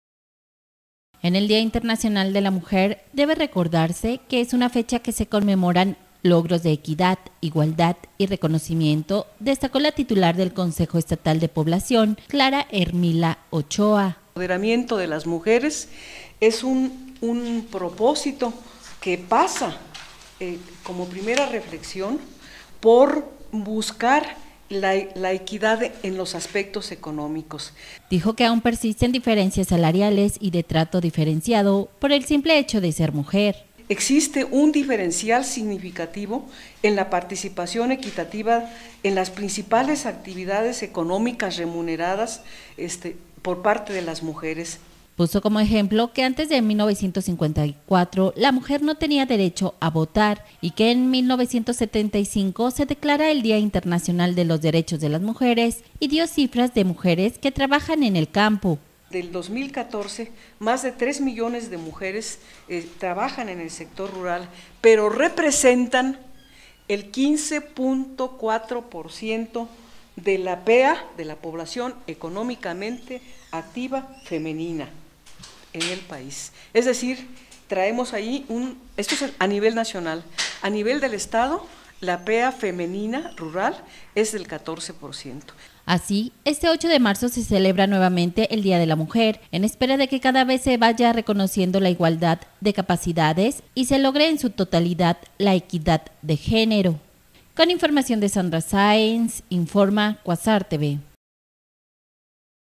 Día de la Mujer, con asignaturas pendientes
En el Día Internacional de la Mujer, debe recordarse que es una fecha que se conmemoran logros de equidad, igualdad y reconocimientos destacó la titular del consejo estatal de población, Clara Hermila Ochoa…